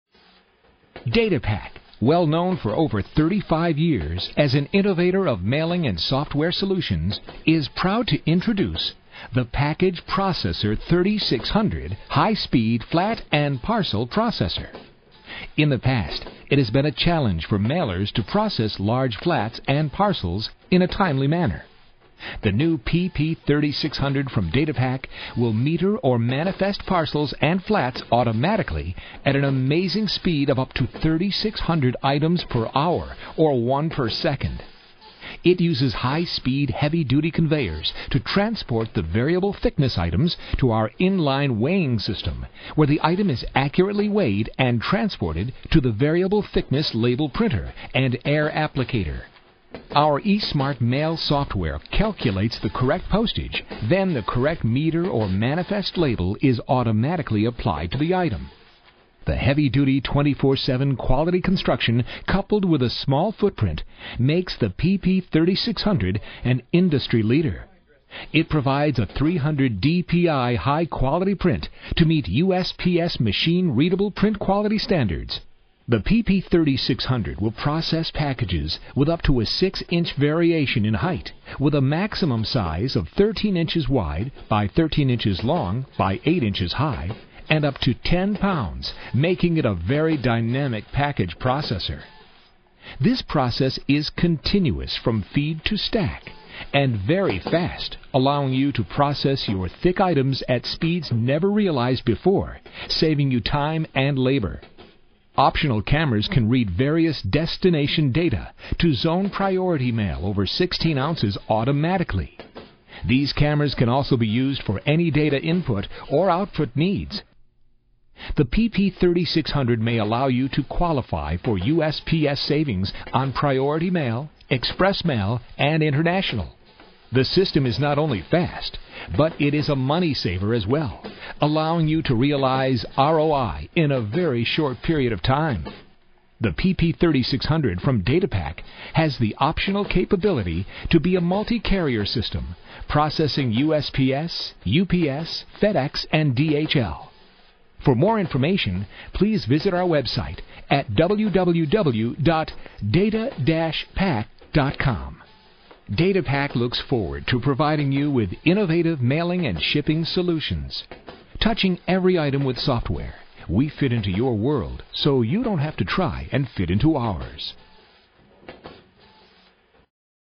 Product Video Demonstration of Data-Pac PP3600 Package Processor
Promotional spot for Data-Pac's new PP3600 - High speed flat and parcel processor.